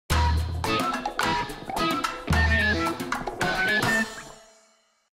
{Without The Voice} PBS Kids Video app startup screen [Lakeland PBS] (13,560th Video)
Credits To PBS Kids & Lakeland PBS